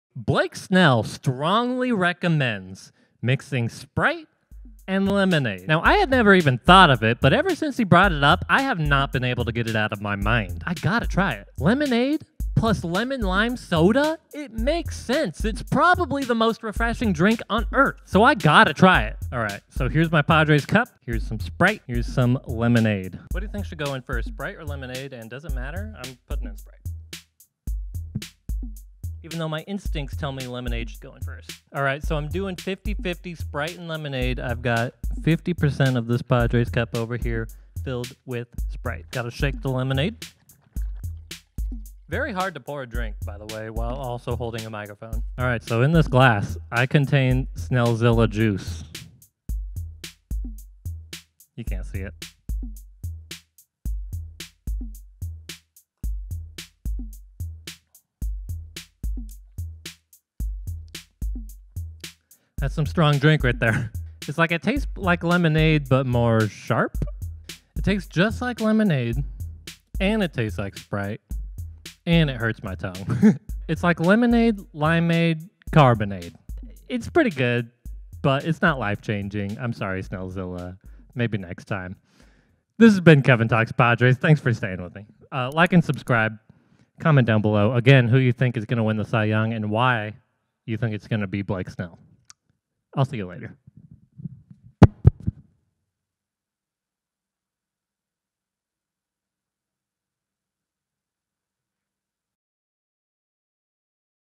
Trying Snellzilla Juice
Blake Snell swears by Lemonade and Sprite mixed together. Sounds good, right? Sounds like the most refreshing drink in the world? Watch me try it to see if it's any good...

Try Snellzilla juice at home!